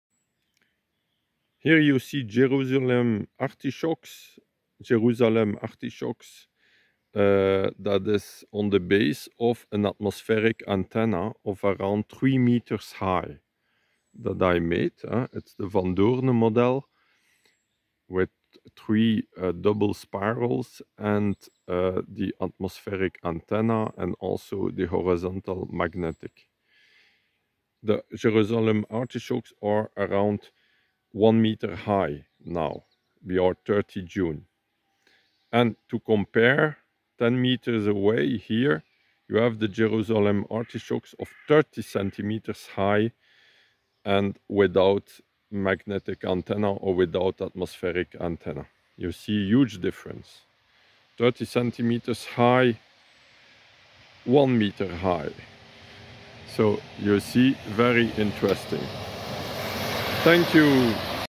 Electroculture on Jerusalem artichokes with an atmospheric antenna Testimonial France - 2023-06-30
At a garden of Yannick Van Doorne, in Alsace.